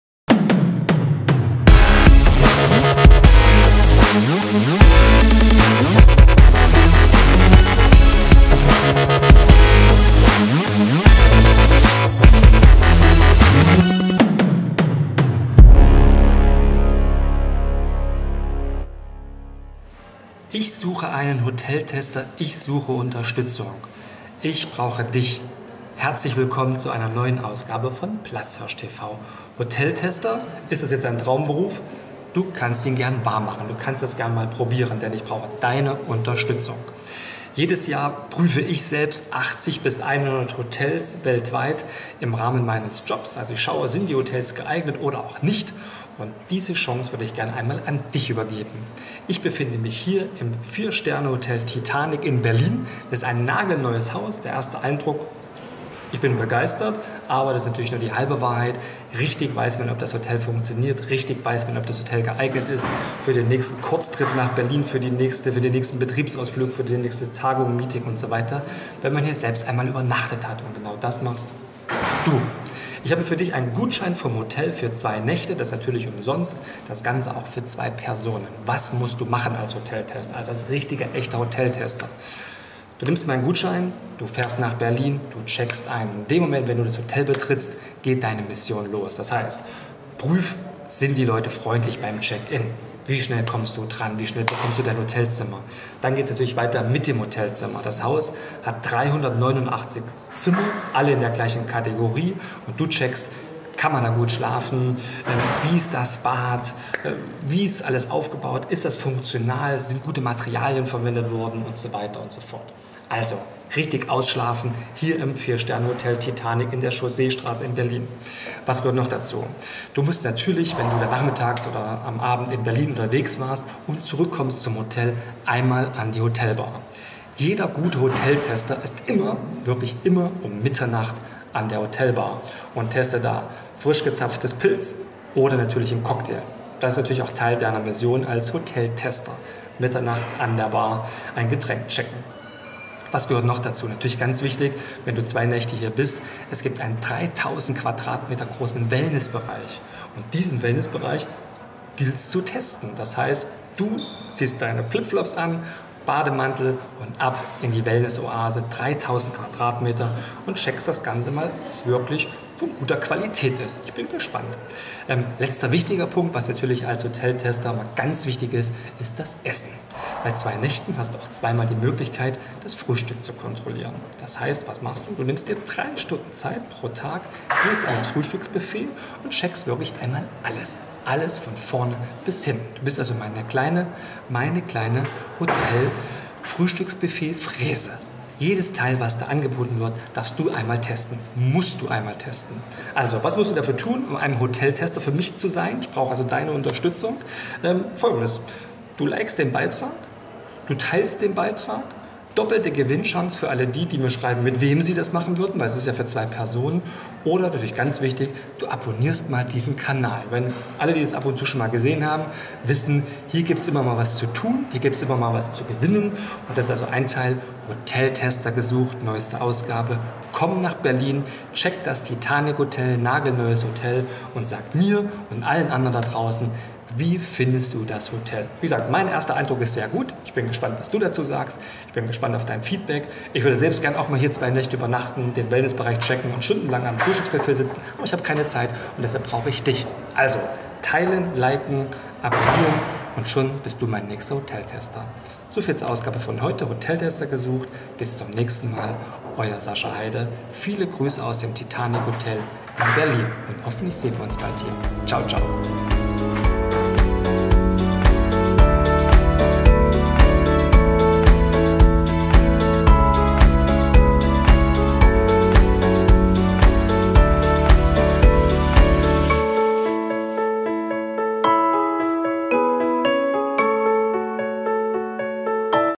Traumberuf Hoteltester - Luxus kostenfrei genießen im Titanic Hotel Berlin (Incentives & Events)
Hoteltester - ein Traumberuf für viele Urlaubs- und Städtereisen-Fans! Hier ist deine Möglichkeit, diesen Traum einmal wahr werden zu lassen! Ich jedenfalls liebe es, als Hoteltester die neuesten Häuser rund um die Welt zu checken. Und jetzt brauche ich deine Unterstützung. Du darfst mit einer Begleitperson für mich das neue 4-Sterne Hotel Titanic in Berlin testen. 2 Nächte, 3 Tage für 2 Personen. Worauf ich achte, wenn ich neue Hotels für Firmen-Events besichtige, erkläre ich dir in diesem Clip.

Sascha Heide ist Experte für Firmen-Events.